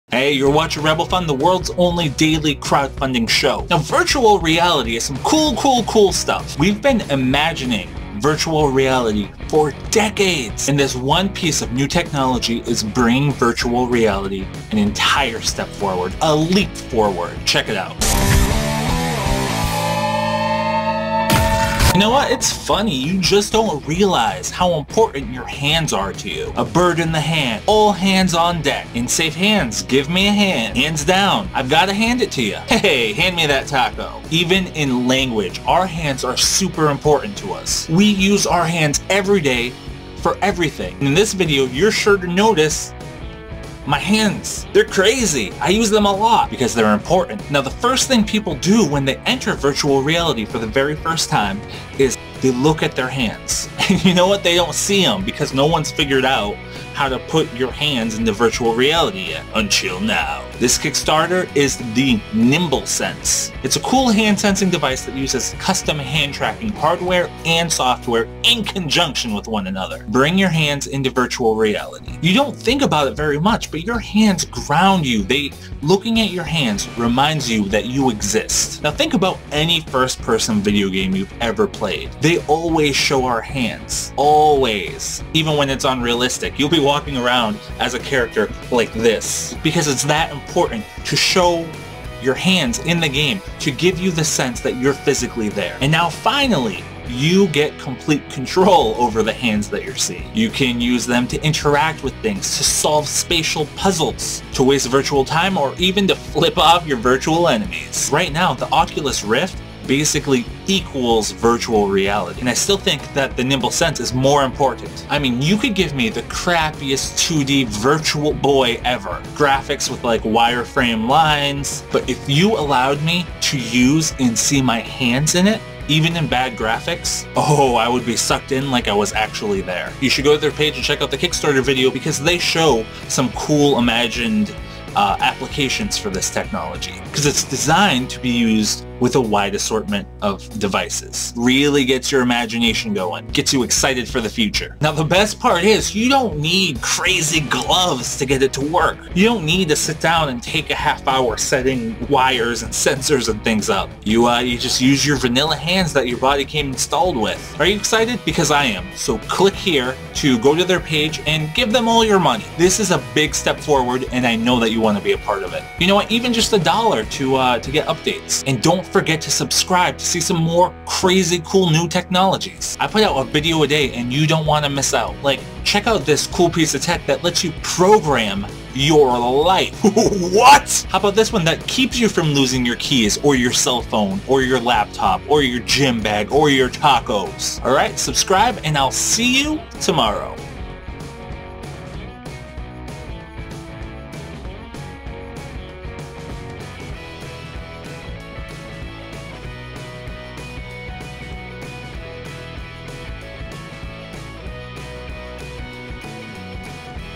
Most Important Advancement in Virtual Reality Ever! Nimble Sense VR
The Nimble Sense is more groundbreaking for Virtual Reality than the Oculus Rift itself! It's a super sensor that allows you to use your  hands as input devices.

The hyper-sensitive pointcloud means instantaneous motion detection of full hand skeletal structures.

This means big things for virtual reality and the way we interact with the real world as well.